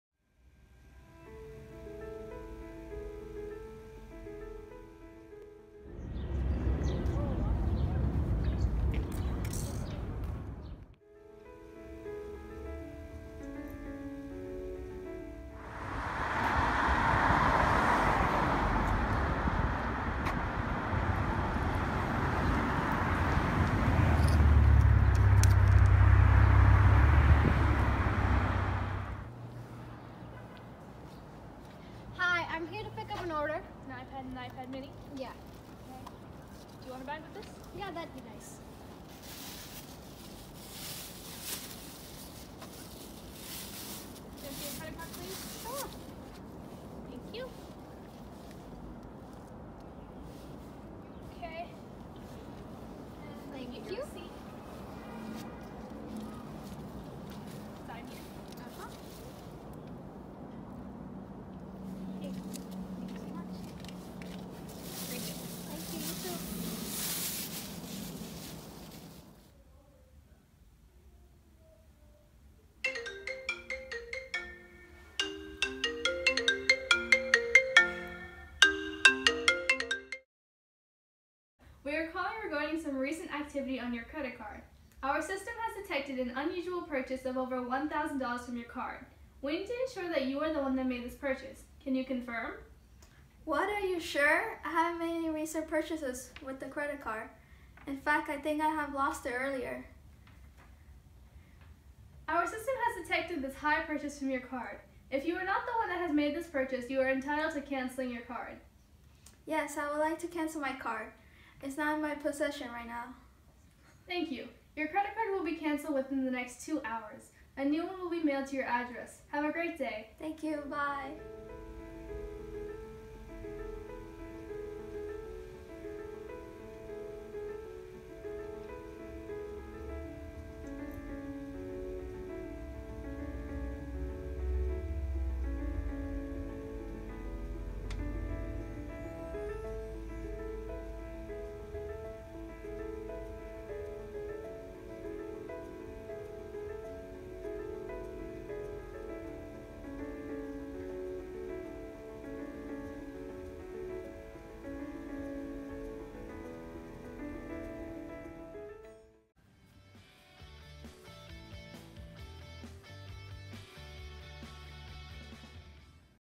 Identity Theft | Student Short Film | PSA | High School (Read Description)
This was the first student short film I made for the Film Studies class I took in high school. I hated this film so much, even just rewatching it to upload is killing me.

It’s so cringy and badly made. I think it’s also a good learning experience because it allowed me to start figuring out how to actually put together better films for the future.

I’m only posting this one for future reference on how far I’ve come, even if this is just a side hobby.

Enjoy? Lol